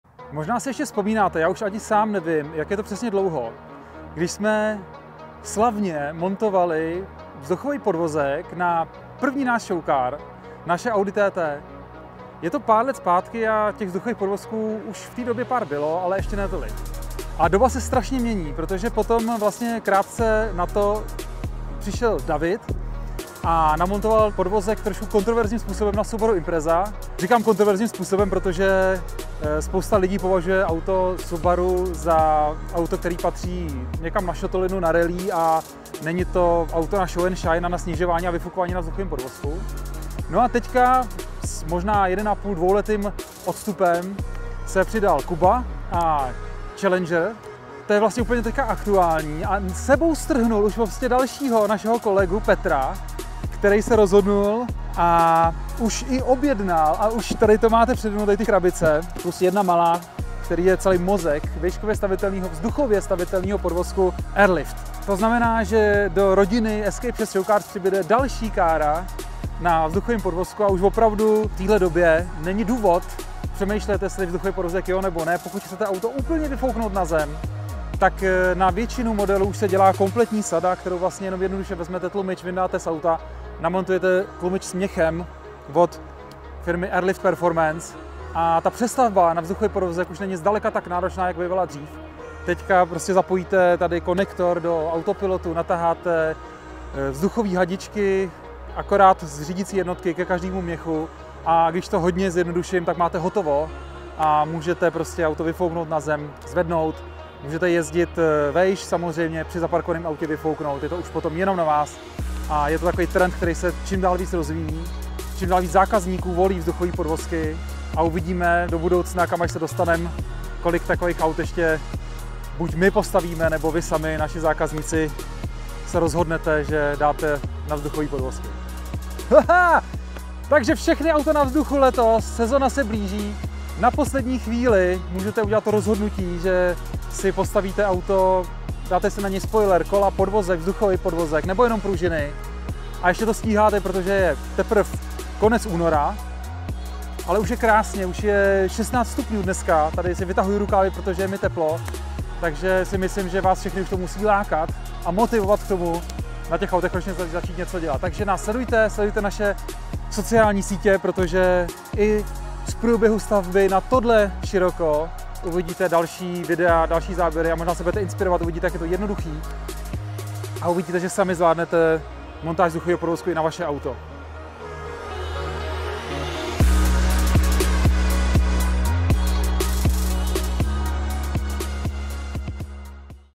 Escape6 VW Scirocco | Air Lift suspension | Výškově stavitelné vzduchové podvozky
Extrémní snižování upravených aut pomocí vzduchových podvozků zažívá nyní skutečný boom. Proč? Protože v současné době je už opravdu jednoduché koupit správné komponenty a doslova za pár dní vše namontovat a zprovoznit. Navíc většina lidí přichází na fakt, že vzduchový podvozek je nejlepší cesta k tomu vůz maximálně snížit pro účely srazů a výstav, ale přitom mu zachovat pohodlí a použitelnost v běžném provozu. Na většinu běžných vozů nabízíme přestavbové sady, takže nemusíte nic vrtat ani řezat. Prostě jen vyměníte originální pružiny a tlumiče za tlumiče osazené vzduchovými měchy, zapojíte ovládání a je to.

Nejnovější přírůstek na vzduchovém podvozku mezi Escape6 showcars bude teď Petrovo Scirocco. Sledujte nás a dozvíte se, jak dopadne montáž a jak bude auto brzo ležet!